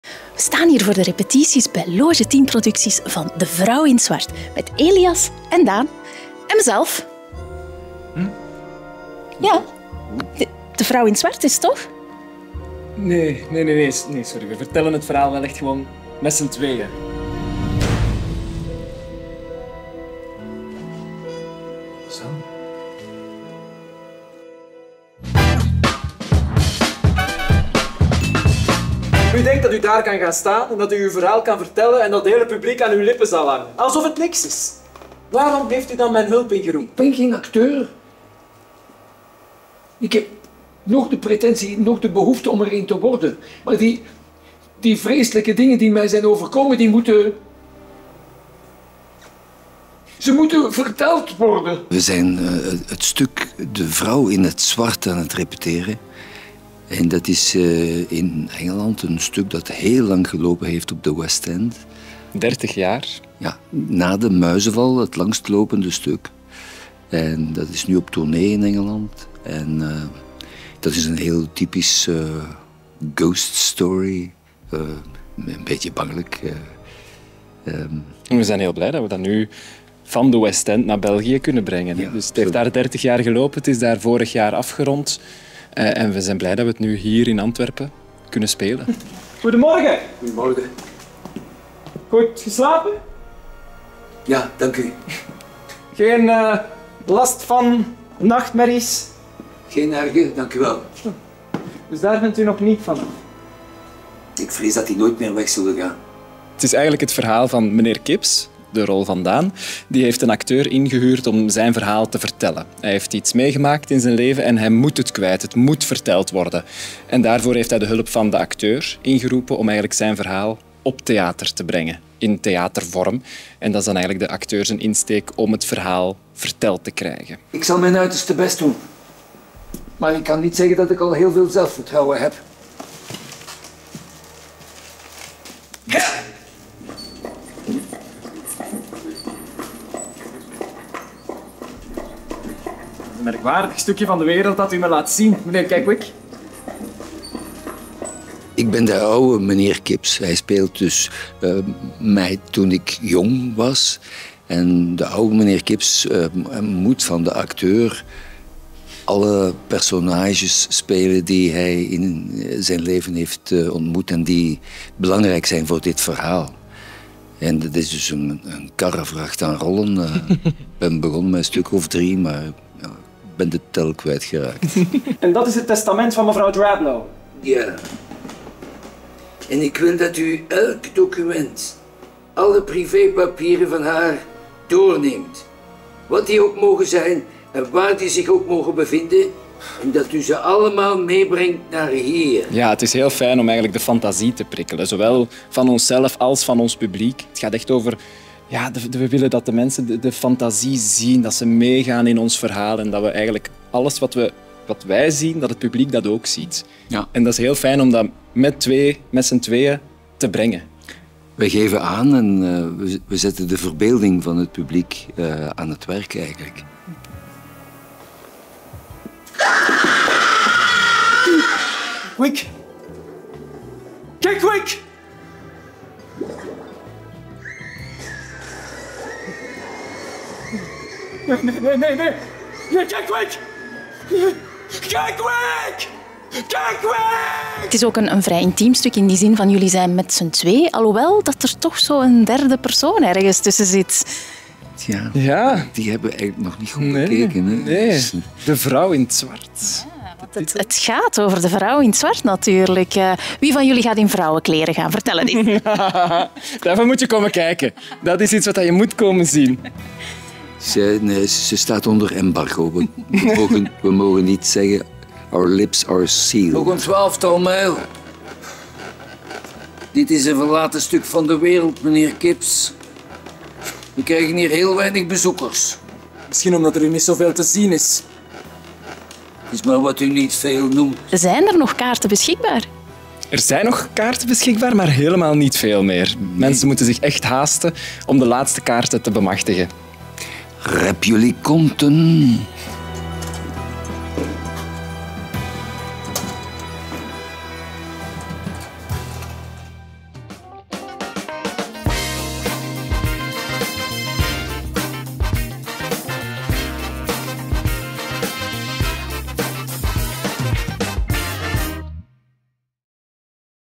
De Vrouw in het zwart - repetities
Binnen minder dan een week gat @loge10theaterproducties44 in première met 'de vrouw in het zwart'.
Eén van de grootste Engelse klassiekers die meer dan 30 jaar op de Londense West End mocht spelen en nu hier in Vlaanderen!

Maak al eens kennis met het verhaal en de cast Elias Bosmans en Daan Hugaert. En wie is nu die vrouw in het zwart?

Vanaf 13 etm 31 maart in de Rode Zaal, Fakkeltheater.

maar zoals Daan zegt: rep jullie konten want de tickets zijn bijna allemaal de deur uit!